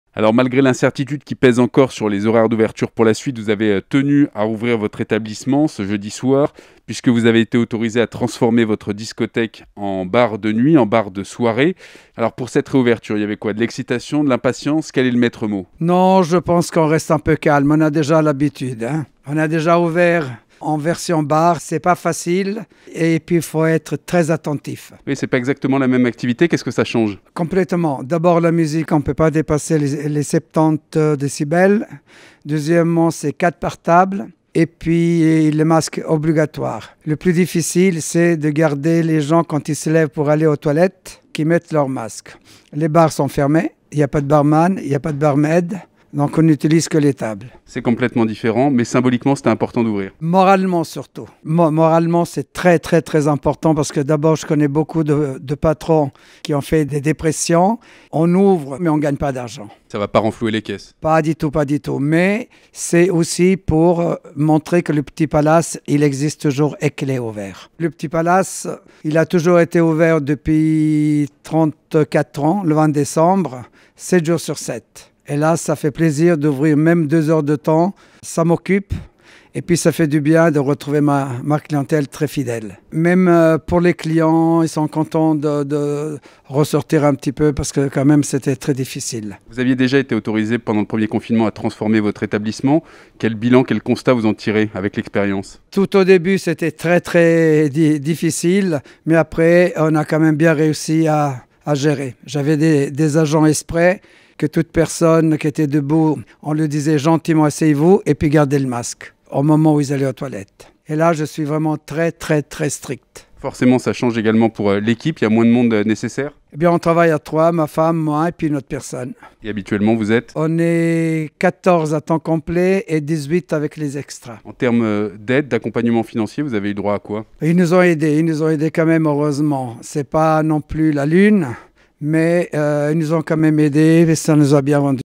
La célèbre discothèque "Le Petit Palace" a été autorisée à rouvrir mais sous forme de bar !
Les nuits genevoises risquent d'être confinées encore un bon petit moment...

Pourtant, la célèbre discothèque genevoise “Le Petit Palace” en vieille-ville, a été autorisée à rouvrir jeudi soir, mais sous forme de bar uniquement, entre 21H et 23H. Une réouverture surtout symbolique pour le patron des lieux, Antoine Macheda.